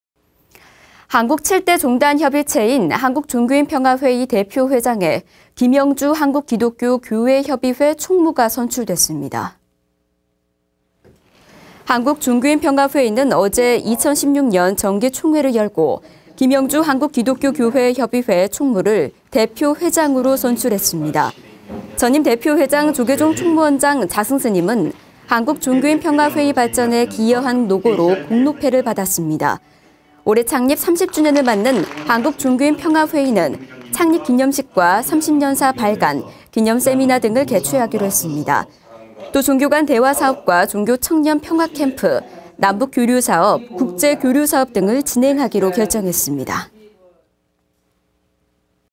[BTN뉴스]한국종교인평화회의 대표회장에 김영주 KNCC총무 선출
한국 7대 종단 협의체인 한국종교인평화회의 대표회장에 김영주 한국기독교교회협의회 총무가 선출됐습니다. 한국종교인평화회의는 어제 2016년 정기.. BTN불교TV | 02-25